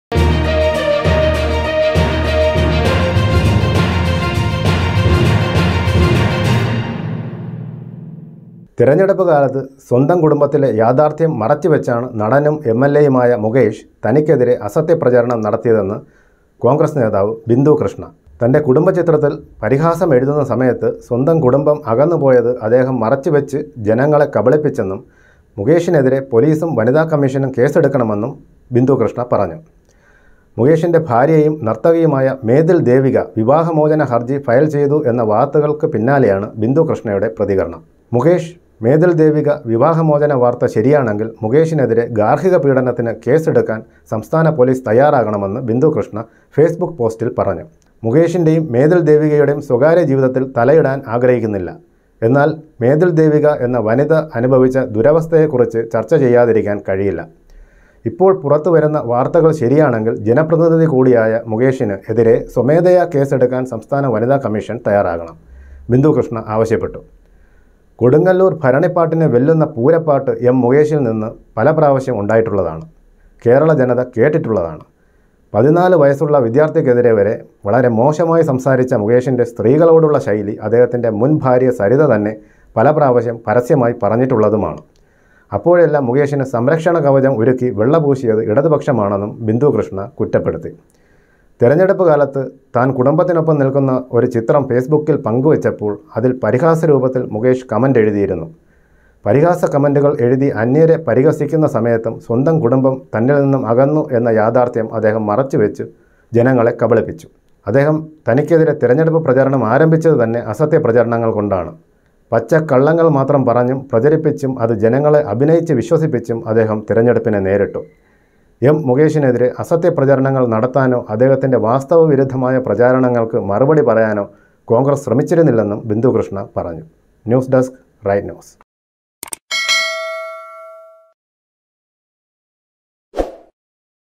മുകേഷിനെ സംരക്ഷിക്കുന്നത് LDF എന്നും ആരോപണം
തെരഞ്ഞെടുപ്പ് കാലത്ത് സ്വന്തം കുടുംബത്തിലെ യാഥാർത്ഥ്യം മറച്ചുവെച്ചാണ് നടനും എംഎൽഎ യുമായ മുകേഷ് തനിക്കെതിരേ അസത്യ പ്രചരണം നടത്തിയതെന്നു കോൺഗ്രസ് നേതാവ് ബിന്ദുകൃഷ്ണ. തന്റെ കുടുംബചിത്രത്തിൽ പരിഹാസം എഴുതുന്ന സമയത്ത് സ്വന്തം കുടുംബം അകന്നുപോയത് അദ്ദേഹം മറച്ചു വെച്ചു ജനങ്ങളെ കബളിപ്പിച്ചെന്നും മുകേഷിനെതിരേ പോലീസും വനിതാകമ്മീഷനും കേസെടുക്കണമെന്നും പറഞ്ഞു.
മുകേഷിന്റെ ഭാര്യയും നിർത്തകിയുമായ മേതിൽ ദേവിക വിവാഹമോചന ഹർജി ഫയൽ ചെയ്തു എന്ന വാർത്തകൾക്ക് പിന്നാലെയാണ് ബിന്ദു കൃഷ്ണയുടെ പ്രതികരണം. മുകേഷ് മേതില് ദേവിക വിവാഹമോചന വാർത്ത ശരിയാണെങ്കിൽ മുകേഷിനെതിരെ ഗാർഹികപീഡനത്തിന് കേസ് എടുക്കാൻ സംസ്ഥാന പൊലീസ് തയ്യാറാകണമെന്ന് ബിന്ദു കൃഷ്ണ ഫേസ്ബുക്ക് പോസ്റ്റിൽ പറഞ്ഞു.